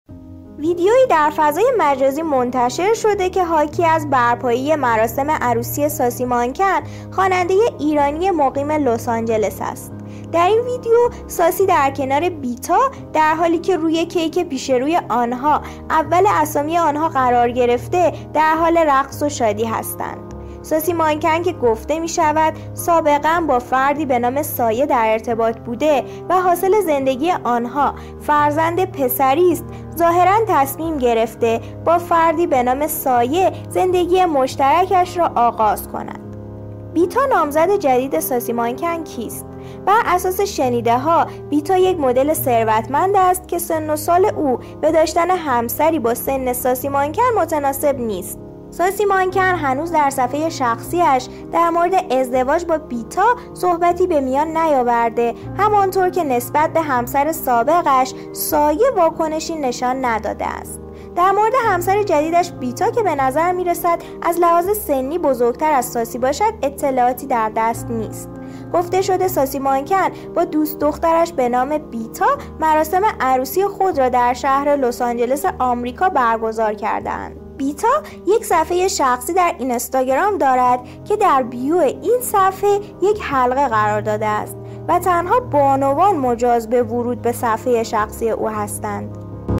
ازدواج مجدد ساسی مانکن با بیتا، مدل پولدار
ویدئویی در فضای مجازی منتشر شده که حاکی از برپایی مراسم عروسی ساسی مانکن خواننده ایرانی مقیم لس آنجلس است .
در این ویدئو ساسی در کنار بیتا در حالی که روی کیک پیش روی آنها اول اسامی آنها قرار گرفته در حال رقص و شادی هستند.
ساسی مانکن که گفته می شود سابقا با فردی به نام سایه در ارتباط بوده و حاصل زندگی آنها فرزند پسری است ، ظاهرا تصمیم گرفته با فردی به نام سایه زندگی مشترکش را آغاز کن